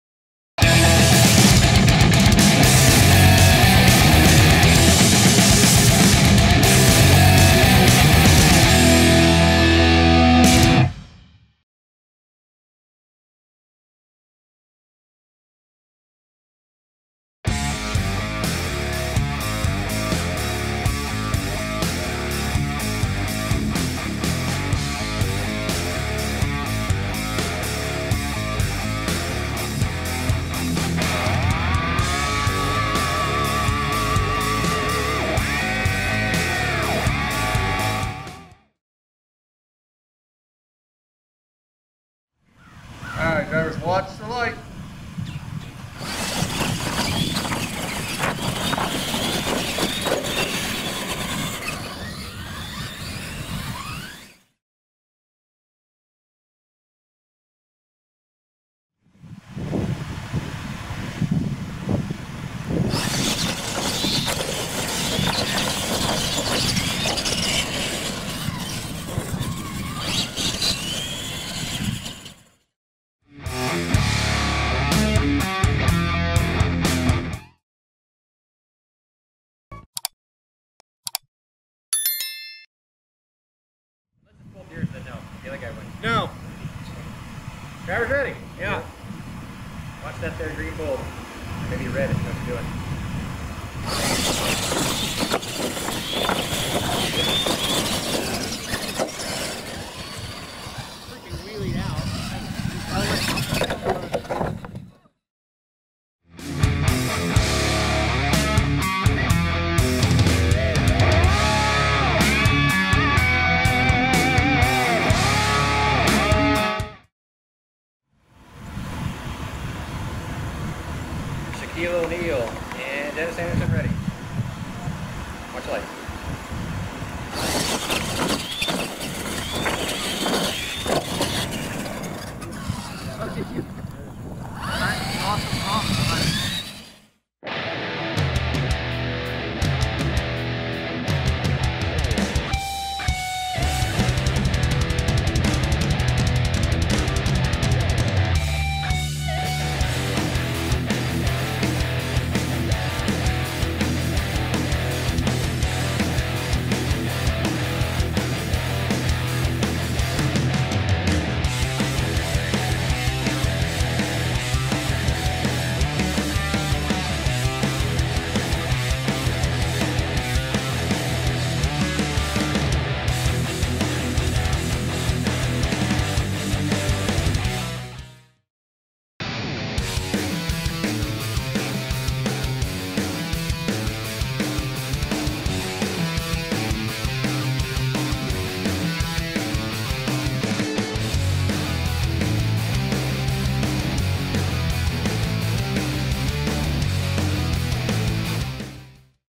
4-WIDE Outlaw Retro Racing - JB Scale Graphics October Madness - Trigger King R/C Monster Trucks
First time ever on TK:  4-WIDE Outlaw Retro racing bracket!  Race is from the JB Scale Graphics October Madness event on October 2, 2021.